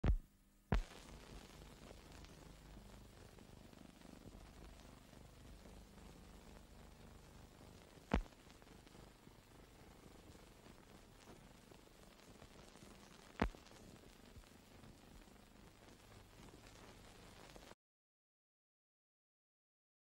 ENGLAND: Lord Wavell leaves to take up position as Viceroy of India (1943)
Lord Wavell leaves to take up position as Viceroy of India.

Full Description:

ENGLAND:
EXT
Lord Archibald Wavell down steps to launch as he embarks to becaome Viceroy of India / Lord and Lady Wavell / lord and Lady Wavell with their daughhter Felicity
Background: Lord Wavell leaves to take up position as Viceroy of India.